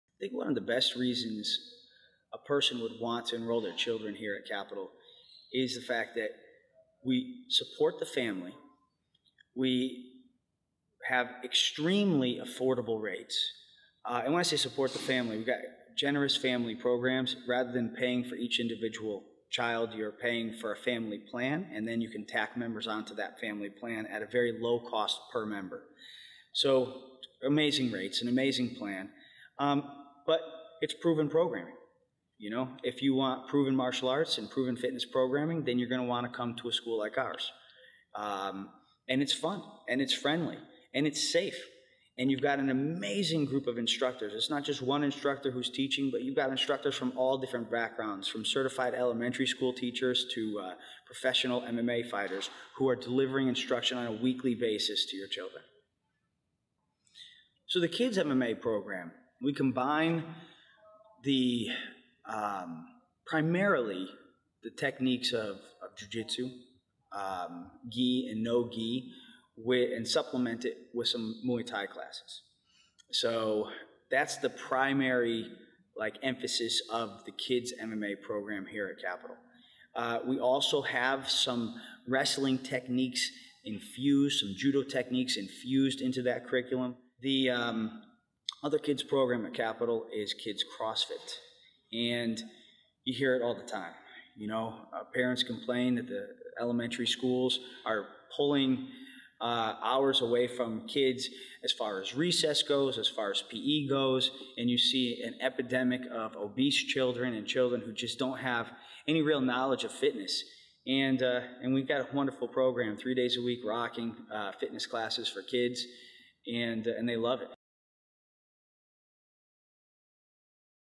Capital MMA & Elite Fitness - Kids Program
Capital MMA prides itself on a long standing reputation for quality instruction in the proven arts. We provide a friendly, safe and fun environment, where children learn to avoid fights, develop positive character traits, learn bully-proofing techniques, and so much more!

We teach Gracie Jiu-Jitsu, Wrestling, Judo, Muay Thai and CrossFit. Give your child:

Self-Discipline
Socialization Skills
Physical Activity
Increased Self-Esteem
A Sense of Respect
Non-Violent Conflict Resolution
Listening Skills
and
Teamwork Skills...